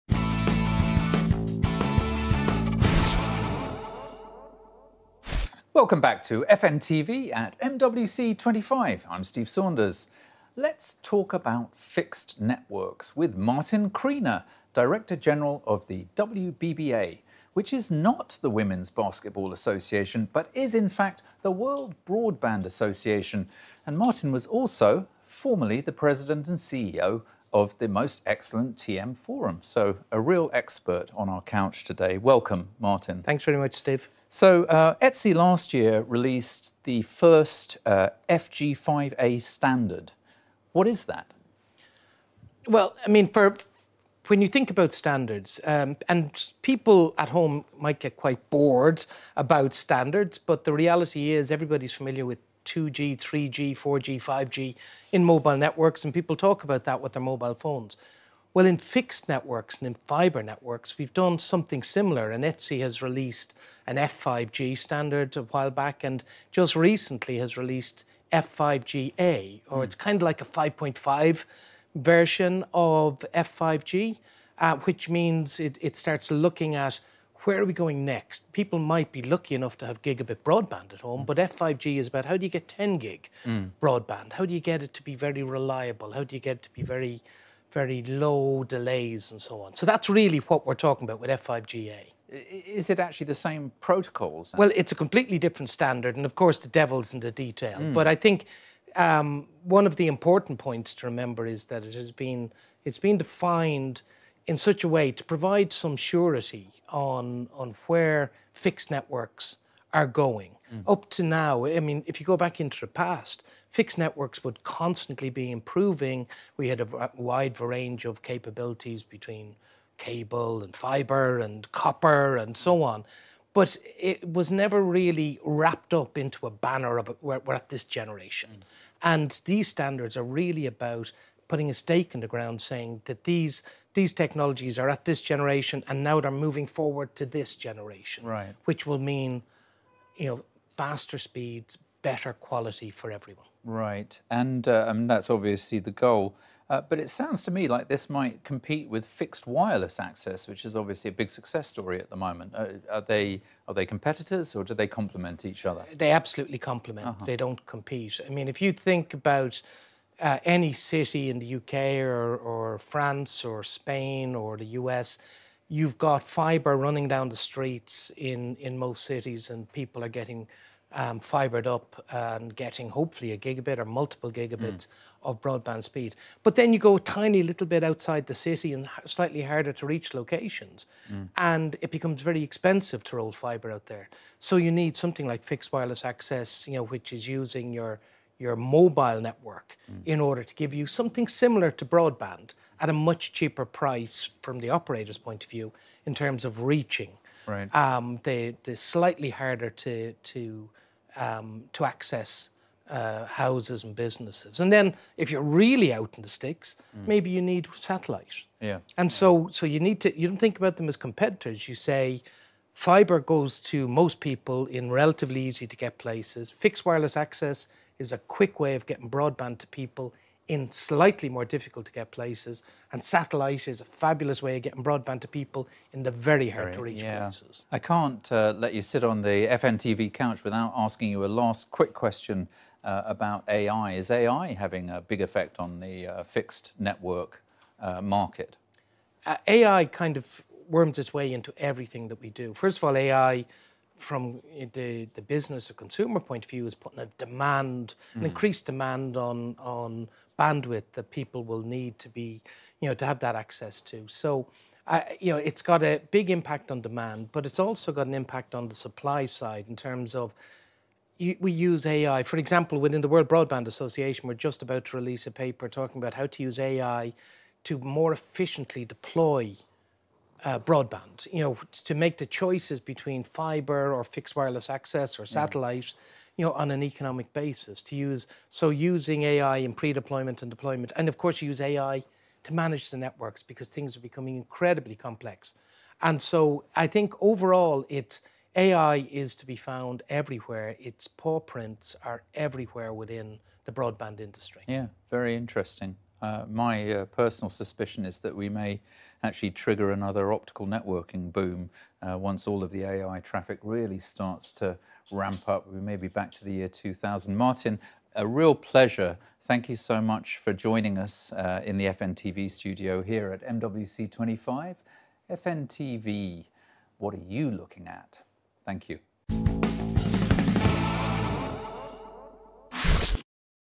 Advancing Fixed Networks: AI-Driven Evolution and Strategic Collaborations
Delve into the significance of ETSI's F5G Advanced Release 3, WBBA's role in promoting next-gen fixed networks, and the transformative impact of AI on industry sustainability and growth.